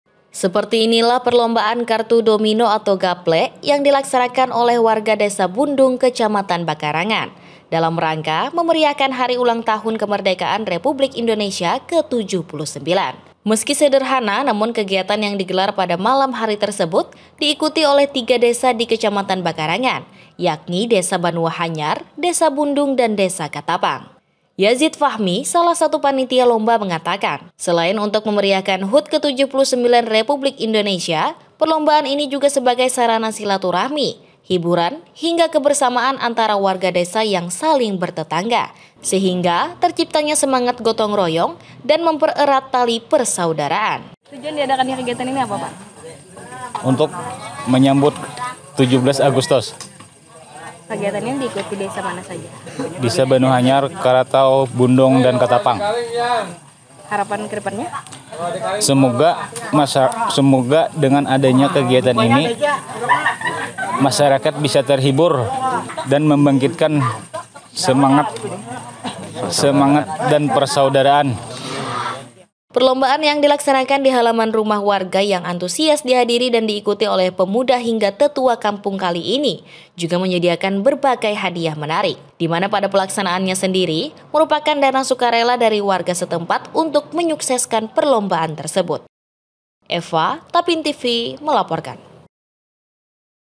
JALIN KEAKRABAN 3 DESA ADAKAN LOMBA GEPLEK
3 DESA DI KECAMATAN BAKARANGAN / IKUTI LOMBA GAPLEK GUNA TINGKATKAN KEAKRABAN SESAMA WARGA // KEGIATAN YANG DIGELAR BERSAMAAN DENGAN HUT RI KE 79 TERSEBUT / ANTUSIAS DIIKUTI PEMUDA HINGGA ORANGTUA //